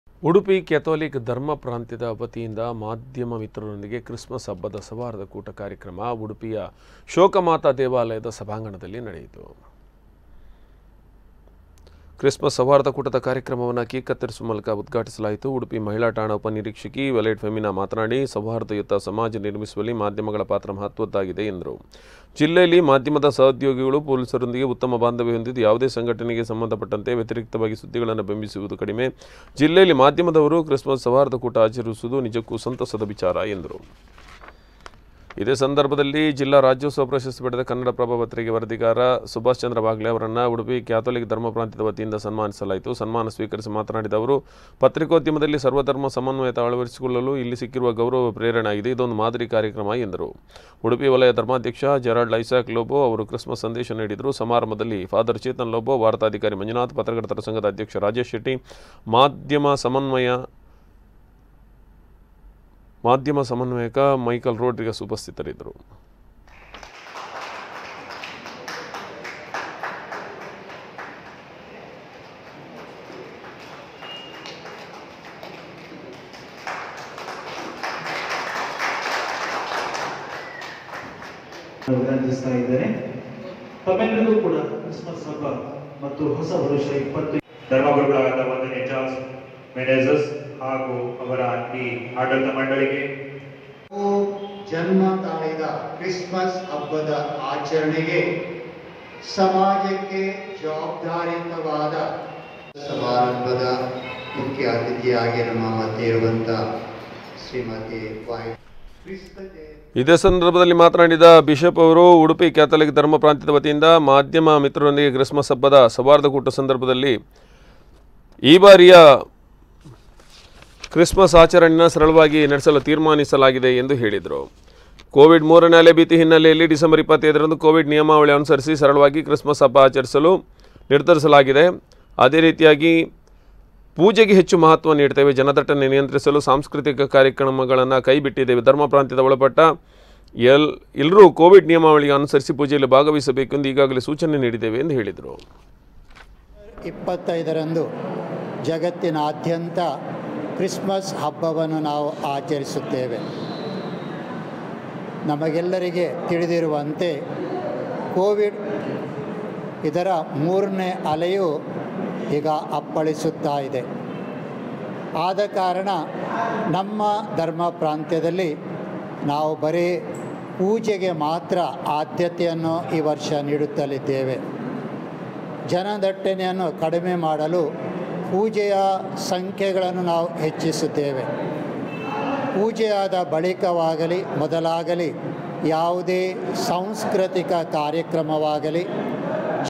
ಉಡುಪಿ ಕ್ಯಾಥೊಲಿಕ್ ಧರ್ಮ ಪ್ರಾಂತ್ಯದ ವತಿಯಿಂದ ಮಾದ್ಯಮ ಮಿತ್ರರೊಂದಿಗೆ ಕ್ರಿಸ್ಮಸ್ ಹಬ್ಬದ ಸೌಹಾರ್ದ ಕೂಟ
ಉಡುಪಿ ಕ್ಯಾಥೊಲಿಕ್ ಧರ್ಮ ಪ್ರಾಂತ್ಯದ ವತಿಯಿಂದ ಮಾದ್ಯಮ ಮಿತ್ರರೊಂದಿಗೆ ಕ್ರಿಸ್ಮಸ್ ಹಬ್ಬದ ಸೌಹಾರ್ದ ಕೂಟ ಕಾರ್ಯಕ್ರಮ ಉಡುಪಿಯ ಶೋಕ ಮಾತಾ ದೇವಾಲಯದ ಸಭಾಂಗಣದಲ್ಲಿ ನಡೆಯಿತು.
ಕ್ರಿಸ್ಮಸ್ ಸೌಹಾರ್ದ ಕೂಟದ ಕಾರ್ಯಕ್ರಮವನ್ನು ಕೇಕ್ ಕತ್ತರಿಸುವ ಮೂಲಕ ಉದ್ಘಾಟಿಸಲಾಯಿತು.
ಉಡುಪಿ ಮಹಿಳಾ ಠಾಣ ಉಪನಿರೀಕ್ಷಕಿ ವೈಲೆಟ್ ಫೆಮಿನ ಮಾತನಾಡಿ ಸೌಹಾರ್ದಯುತ ಸಮಾಜ ನಿರ್ಮಿಸುವಲ್ಲಿ ಮಾದ್ಯಮಗಳ ಪಾತ್ರ ಮಹತ್ವದ್ದಾಗಿದೆ. ಜಿಲ್ಲೆಯಲ್ಲಿ ಮಾಧ್ಯಮದ ಸಹೊದ್ಯೋಗಿಗಳು ಪೊಲೀಸರೊಂದಿಗೆ ಉತ್ತಮ ಬಾಂಧವ್ಯ ಹೊಂದಿದ್ದು ಯಾವುದೇ ಘಟನೆಗೆ ಸಂಬAಧಪಟ್ಟAತೆ ವ್ಯತಿರಿಕ್ತವಾಗಿ ಸುದ್ದಿಗಳನ್ನು ಬಿಂಬಿಸುವುದು ಕಡಿಮೆ . ಜಿಲ್ಲೆಯಲ್ಲಿ ಮಾದ್ಯಮದವರು ಕ್ರಿಸ್ಮಸ್ ಸೌಹಾರ್ದ ಕೂಟ ಆಚರಿಸುವುದು ನಿಜಕ್ಕೂ ಸಂತಸದ ವಿಚಾರ ಎಂದರು.